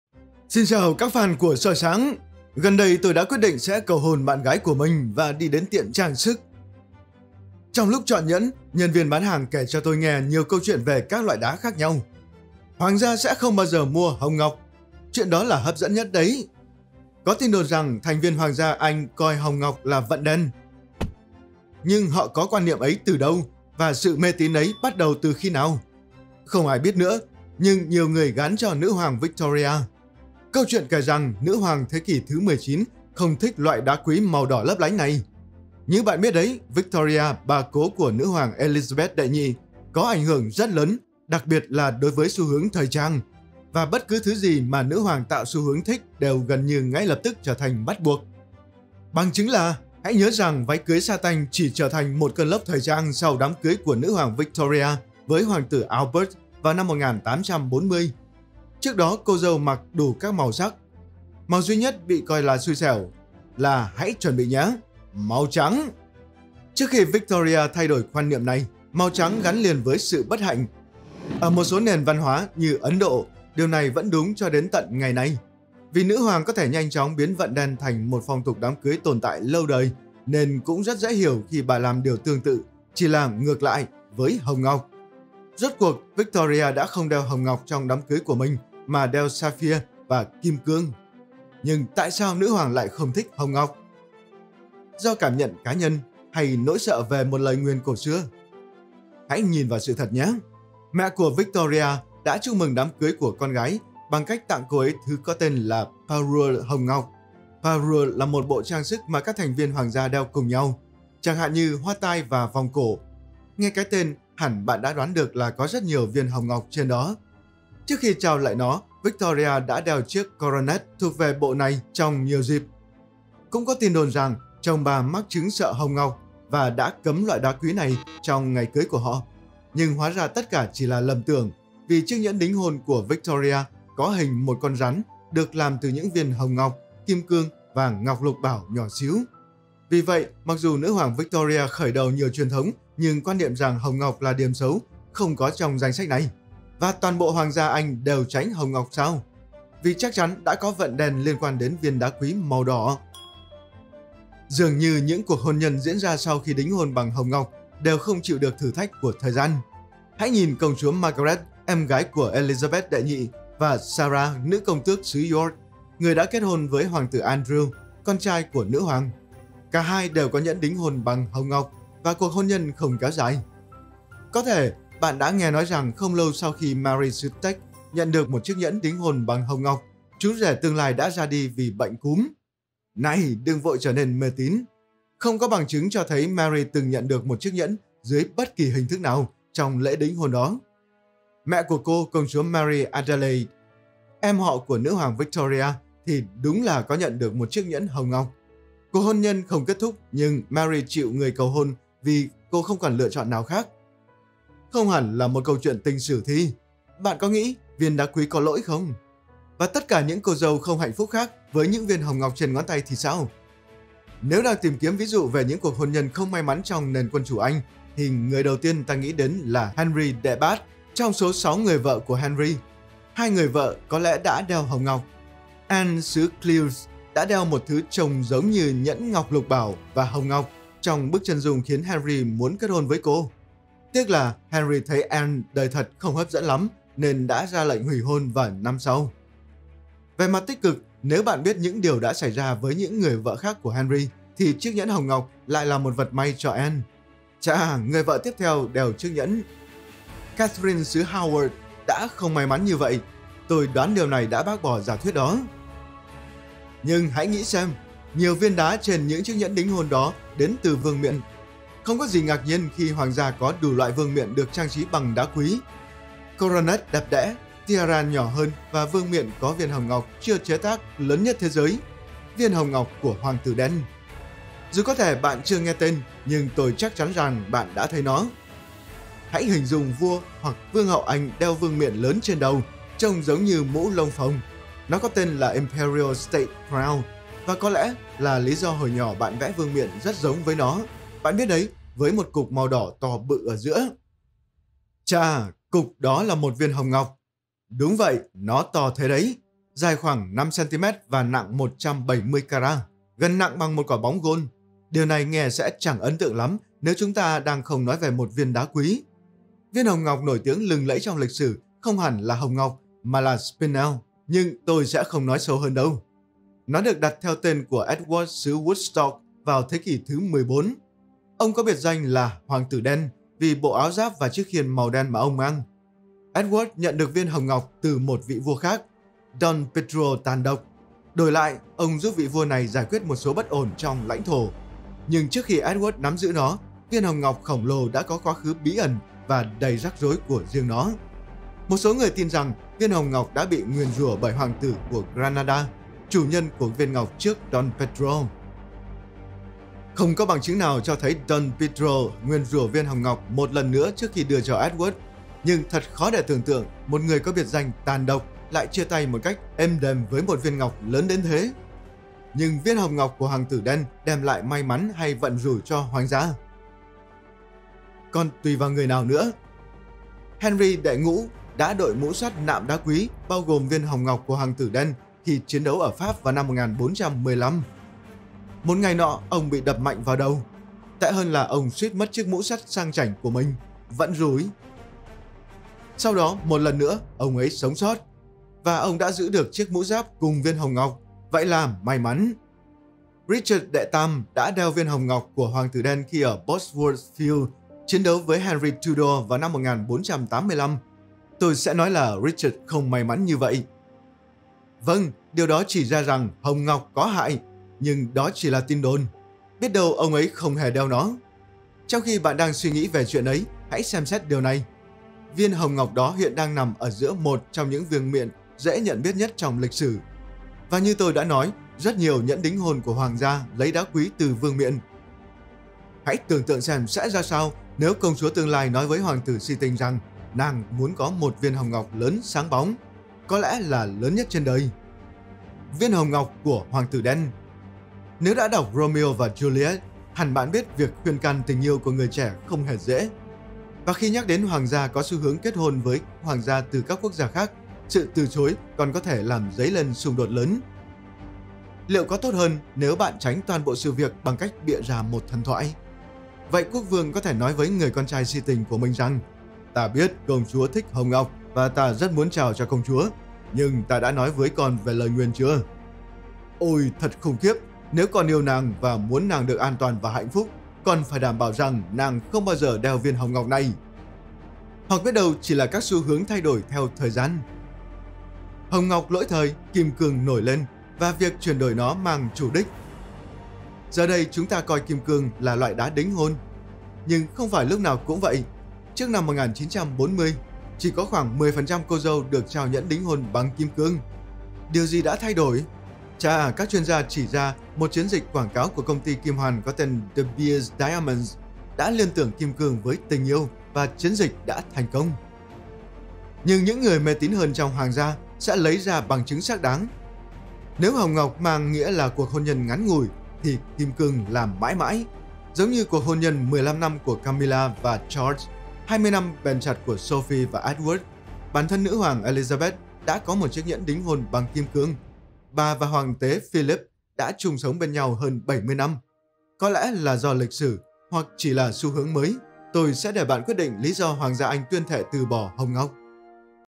Câu Chuyện Bí Ẩn Về Viên Đá Quý Đen Đủi Nhất Đối Với Hoàng Gia Anh
Tất cả các loại đá quý đều đẹp, nhưng hồng ngọc thì quá đỗi mê say. Chúng có màu đỏ và lấp lánh. Trong nhiều thế kỷ, mọi người đã chọn chúng làm một món quà đặc biệt và đeo trong những dịp rất đặc biệt. Tuy nhiên, cũng có đủ những câu chuyện về những viên hồng ngọc mang lại điều xui xẻo và nguy hiểm cho mạng sống của một người. Hồng ngọc có thực sự có những đặc tính đặc biệt như một số người vẫn tin không?

Hoàng gia Anh thích đeo tất cả các loại đá: kim cương, sapphire, ngọc lục bảo. Nhưng bạn không thể nhìn thấy một thành viên trong hoàng gia đeo một viên hồng ngọc. Trong video này, tôi sẽ giải thích nguồn gốc của truyền thống này cũng như hoàng gia nào cấm hồng ngọc ​​và nói rằng chúng mang lại vận rủi.

Sarah, Đám cưới nữ công tước xứ York: Bởi Elke Wetzig
Hoàng tử Edward, Bá tước xứ Wessex và Sophie: Bởi Frankie Fouganthin

Video được tạo bởi Soi Sáng.